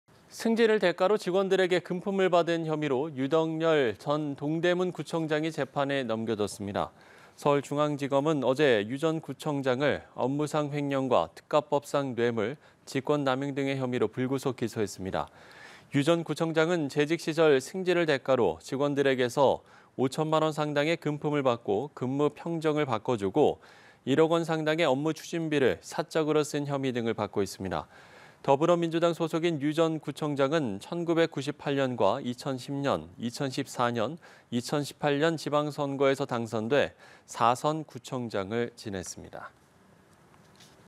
검찰, 유덕열 전 동대문구청장 기소…뇌물·횡령 등 혐의 / 연합뉴스TV (YonhapnewsTV)
검찰, 유덕열 전 동대문구청장 기소…뇌물·횡령 등 혐의

승진을 대가로 직원들에게 금품을 받은 혐의로 유덕열 전 동대문구청장이 재판에 넘겨졌습니다.

서울중앙지검은 어제(13일) 유 전 구청장을 업무상 횡령과 특가법상 뇌물, 직권남용 등의 혐의로 불구속 기소했습니다.

유 전 구청장은 재직 시절 승진을 대가로 직원들에게서 5천만 원 상당의 금품을 받고 근무 평정을 바꿔주고, 1억 원 상당의 업무추진비를 사적으로 쓴 혐의 등을 받고 있습니다.

더불어민주당 소속인 유 전 구청장은 1998년과 2010년·2014년·2018년 지방선거에서 당선돼 '4선 구청장'을 지냈습니다.